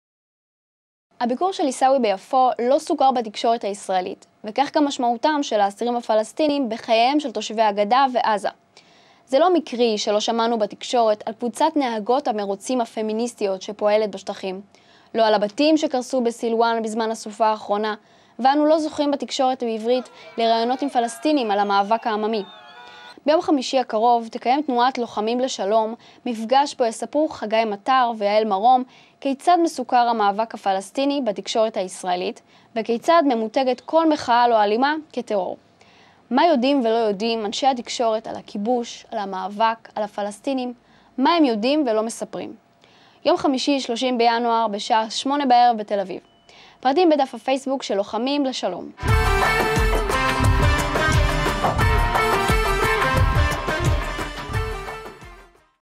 כיבוש ותקשורת
זה לא מקרי שלא שמענו בתקשורת על קבוצת נהגות המרוצים הפמיניסטיות שפועלת בשטחים, לא על הבתים שקרסו בסילוואן בזמן הסופה האחרונה ואנו לא זוכים בתקשורת בעברית לראיונות עם פלסטינים על המאבק העממי. ביום חמישי הקרוב (30/1/2014) תקיים תנועת "לוחמים לשלום" מפגש בו יספרו חגי מטר ויעל מרום כיצד מסוקר המאבק הפלסטיני בתקשורת הישראלית, וכיצד ממותגת כל מחאה לא אלימה כטרור. מה יודעים ולא יודעים אנשי התקשורת על הכיבוש, על המאבק, על הפלסטינים, מה הם יודעים ולא מספרים.